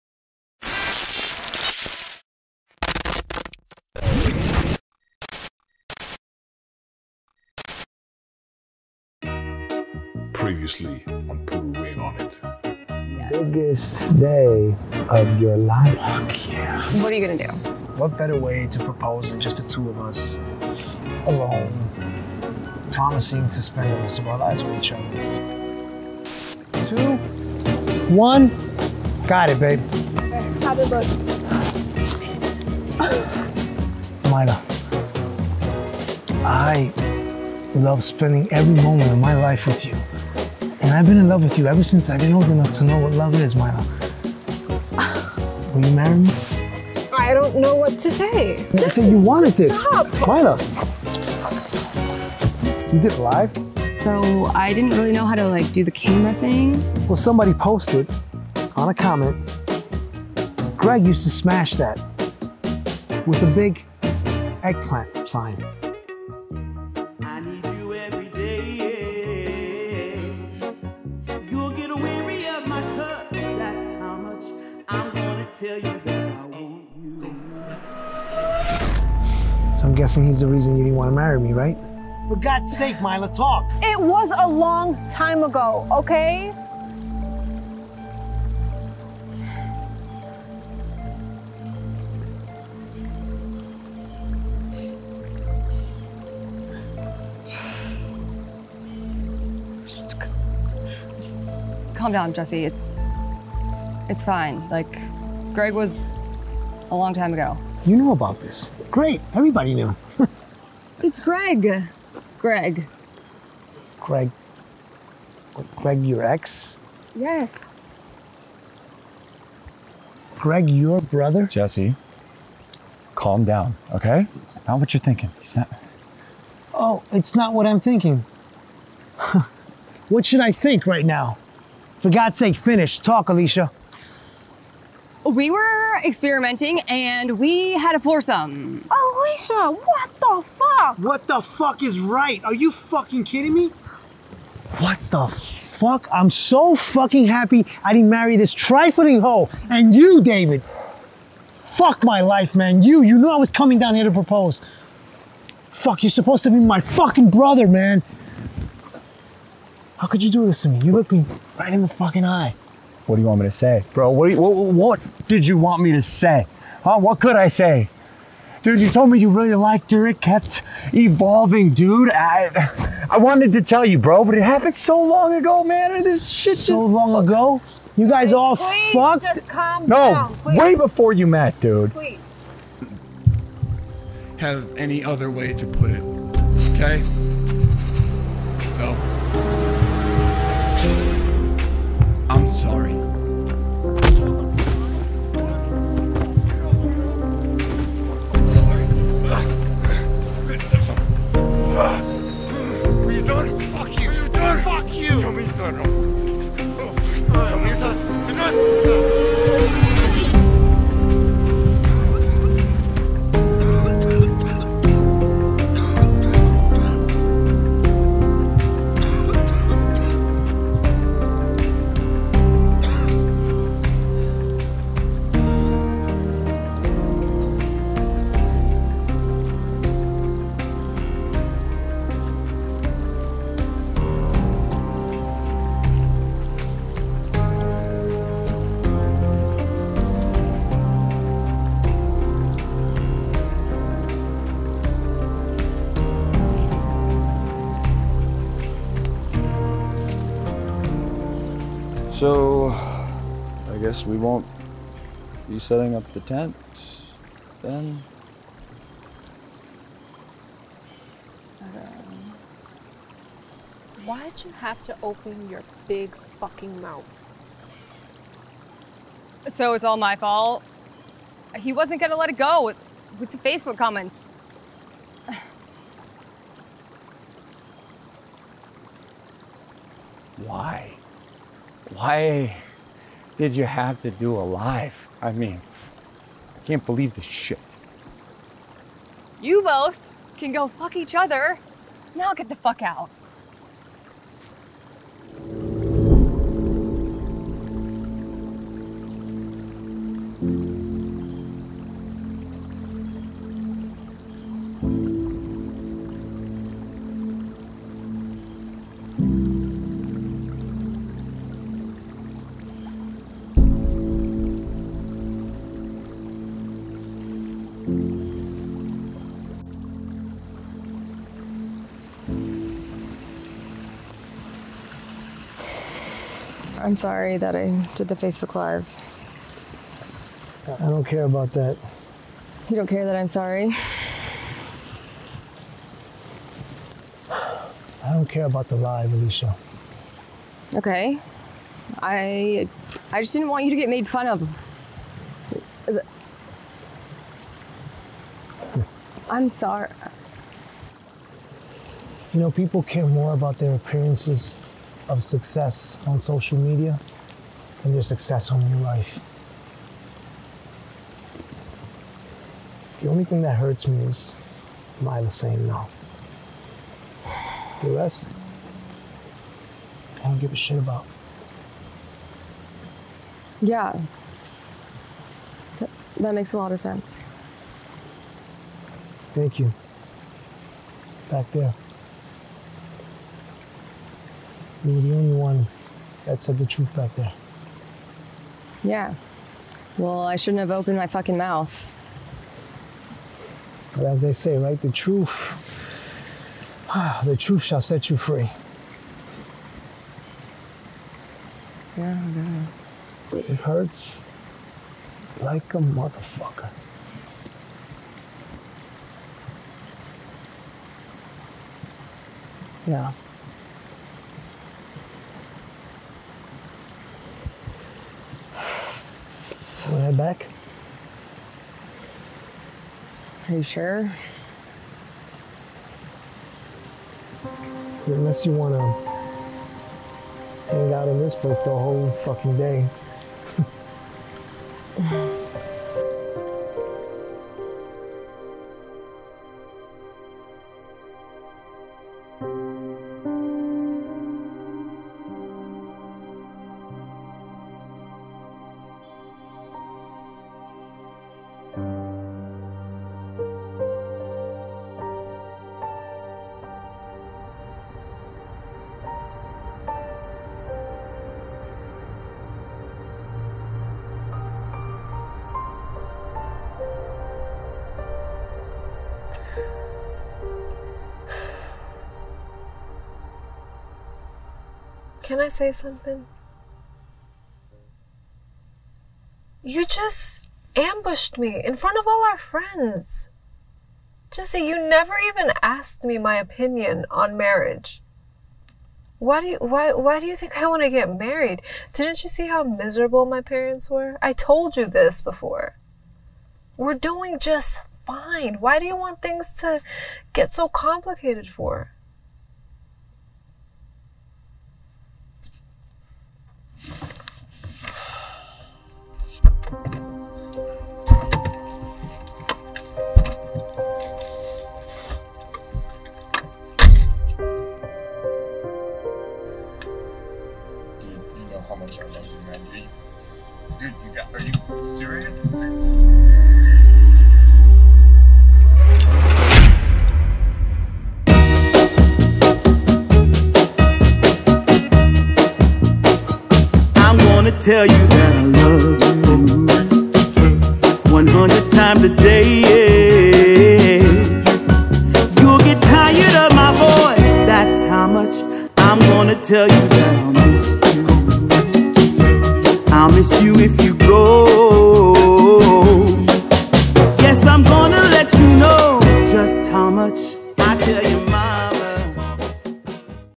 PUT A RING ON IT - EPISODE 3
Today Jessse decides to surprise his girlfriend Maila by taking her and their 2 respective best friends on a road trip to the forest and propose.

Once they arrive on location he gets on his knee and pop the question while the 2 best friends are filming and taking photos, but nothing goes as expected.